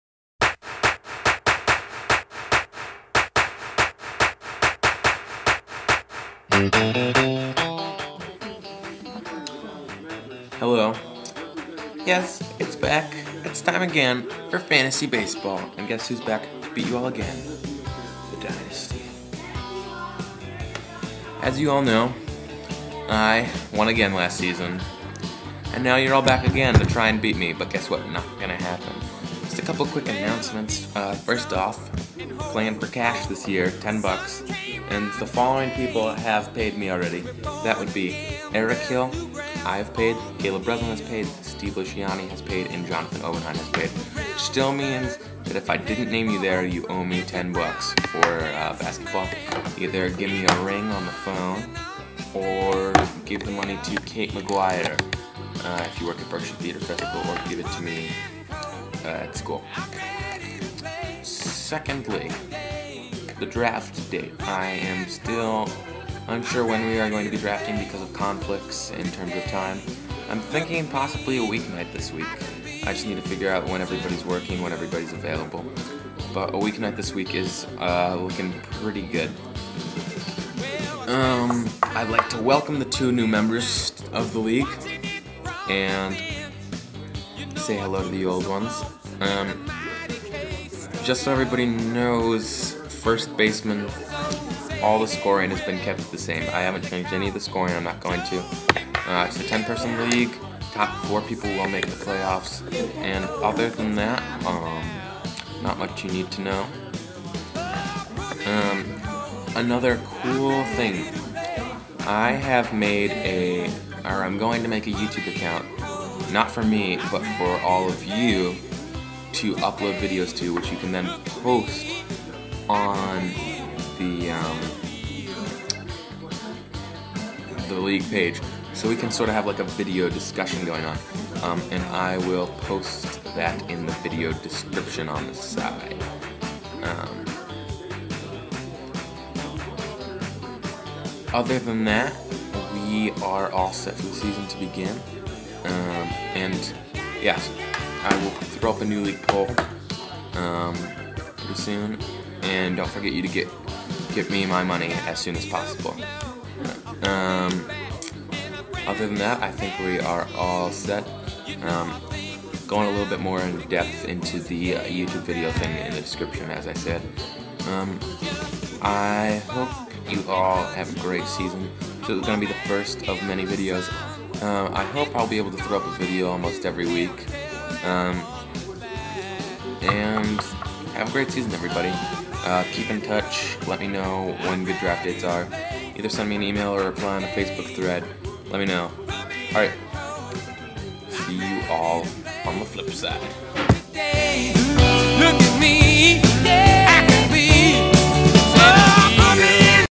Fantasy Baseball Preseason Update
The first video installment of the 2010 baseball season.  Info for the youtube account will be on the league page in a thread.